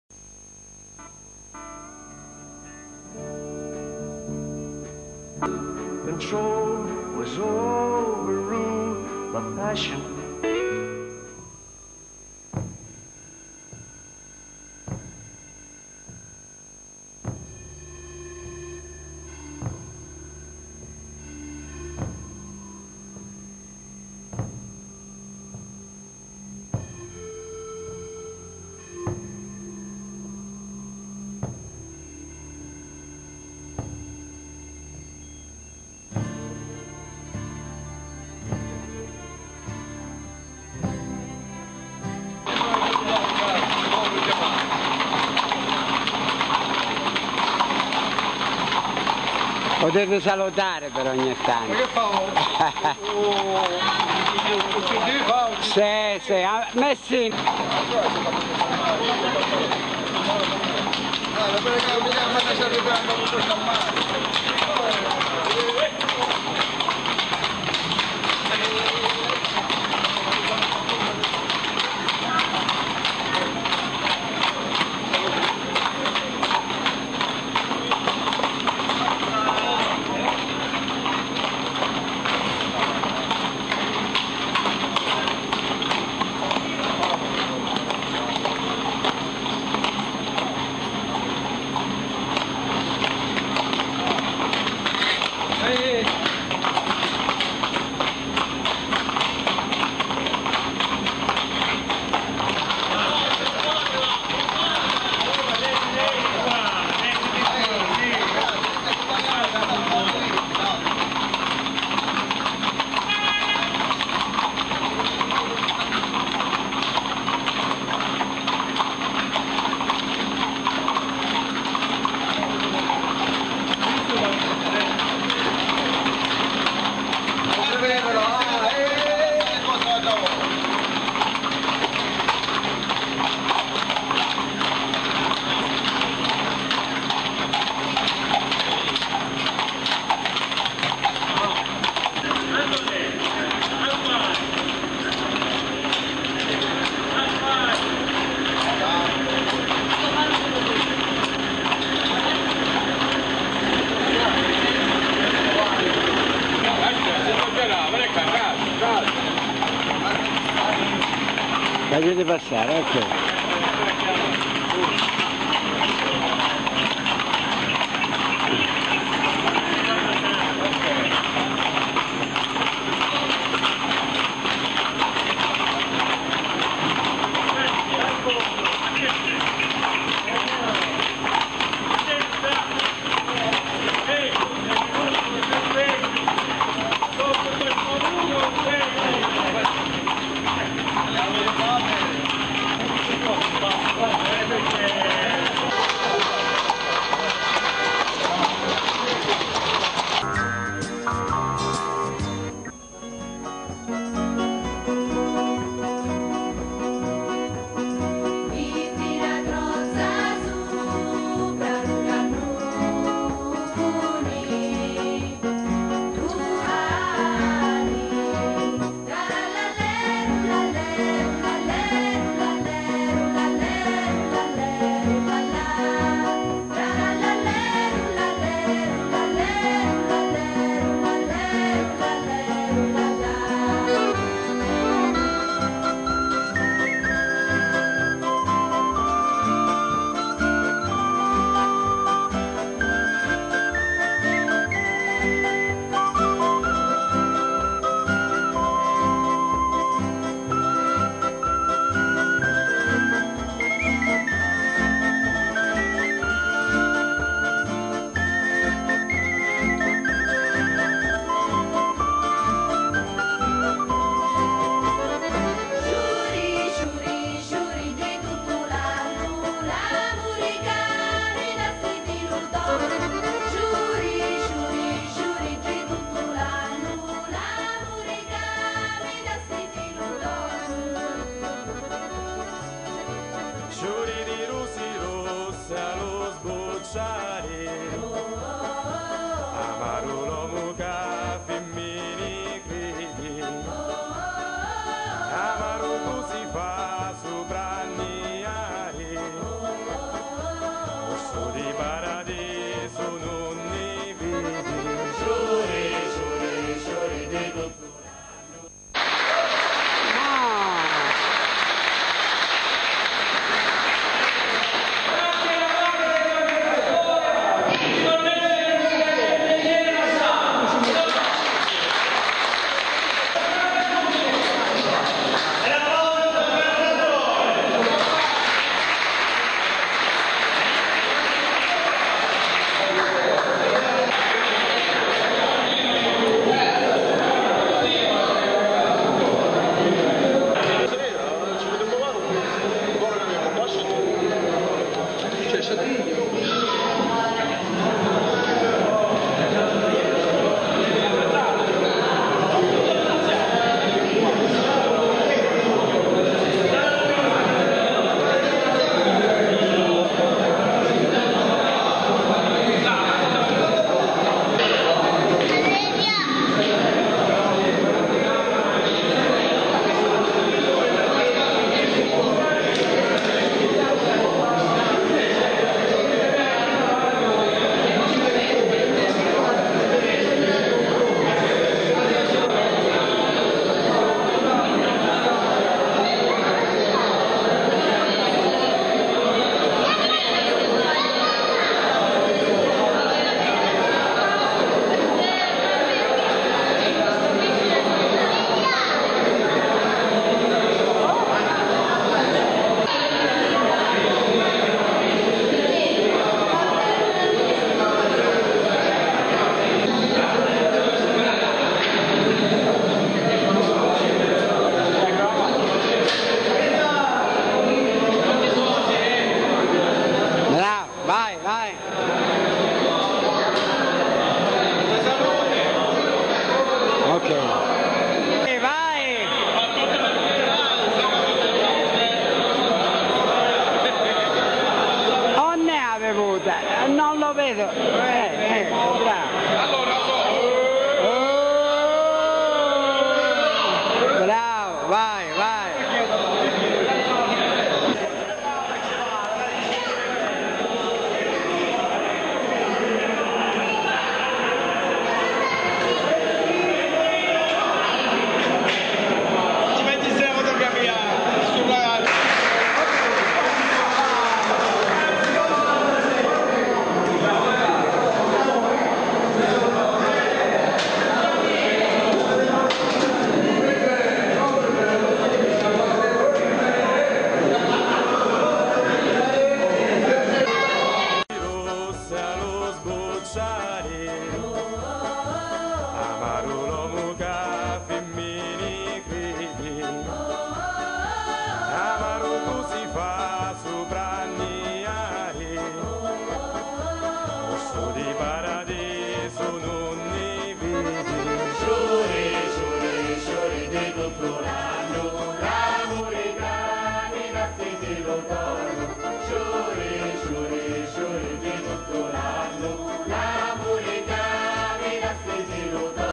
Trekking a cavallo S Stefano Quisquina
Trekking a cavallo organizzato dall'Associazione equestre "I cavalieri della Quisquina" nei giorni 28 e 29 Giugno 2008.